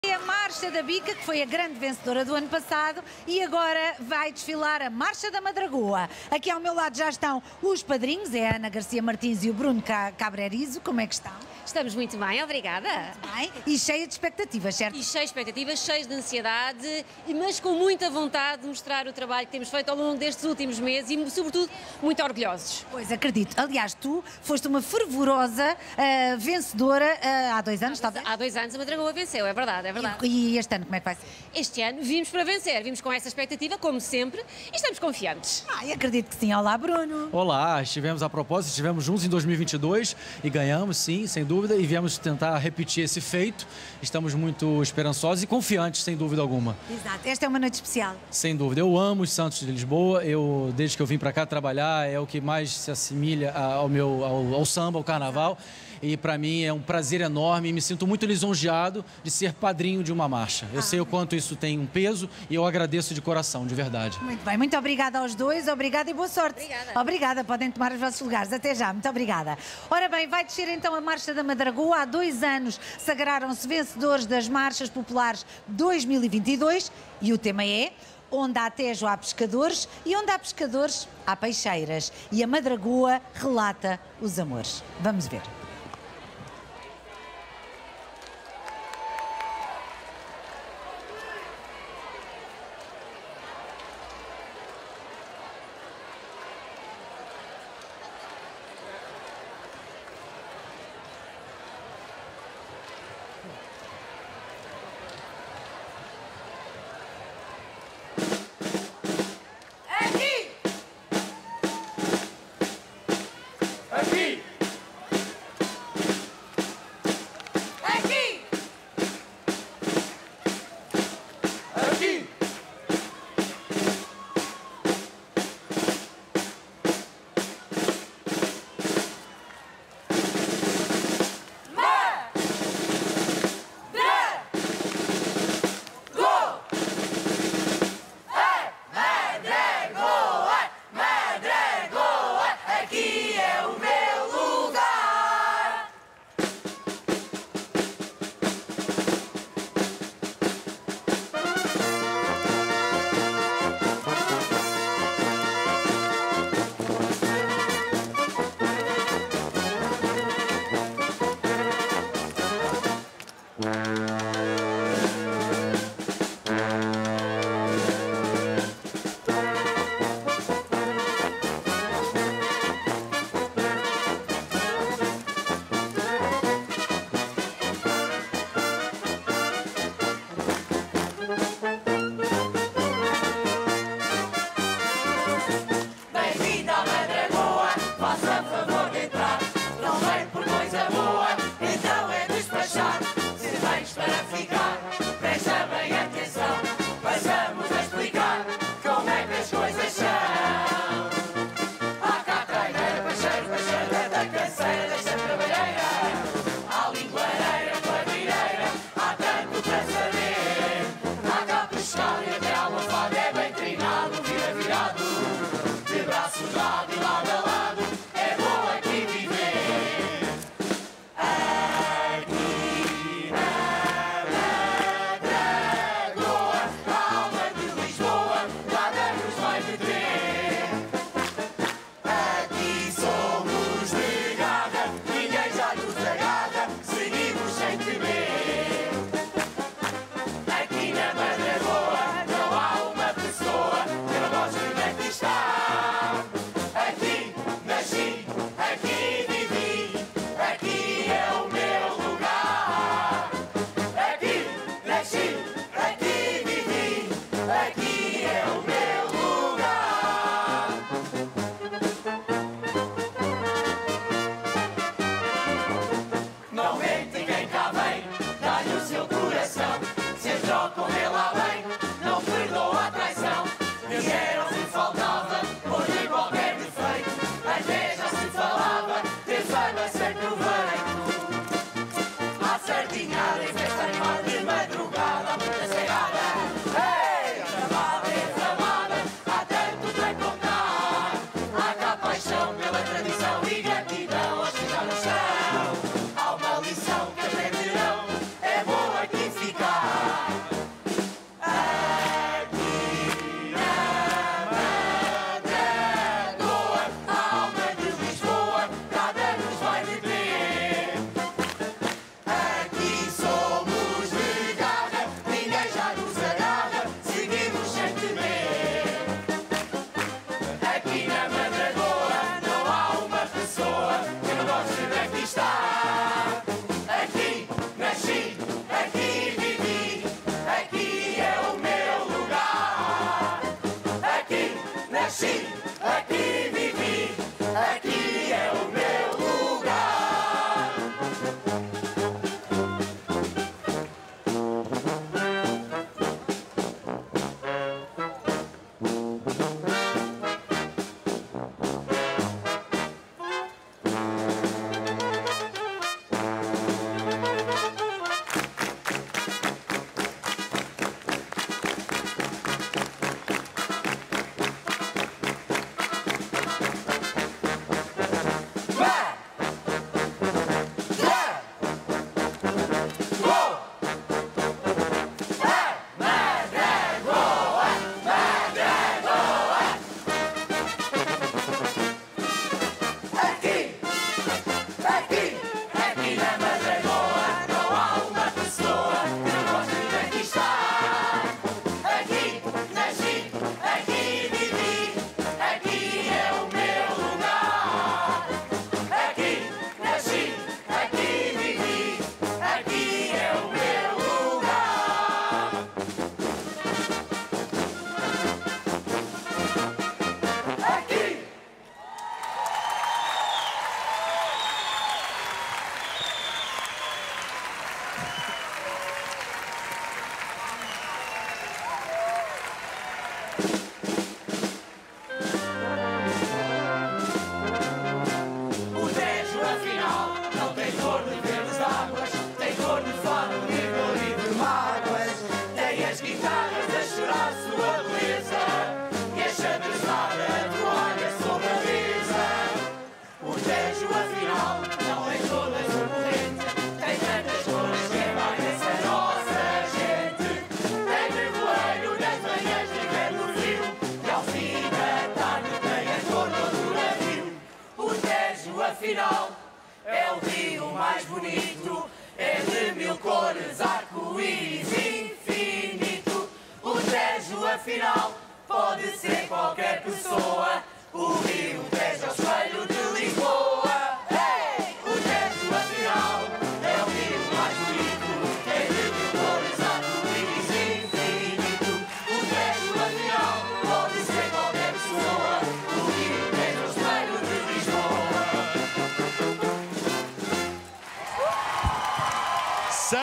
MARCHA DA MADRAGOA 2024 - AVENIDA
6° Lugar das Marchas Populares de Lisboa 2024: 🥁

Tema - 'O meu coração é teu'

Padrinhos: Bruno Cabrerizo e Ana Garcia Martins
Ensaiador: João Medeiros
Figurinista: Fauze El Kadre e João Medeiros
Cenógrafo: Fauze El Kadre e João Medeiros

Transmissão RTP1 🎥

Marchas Populares ✍️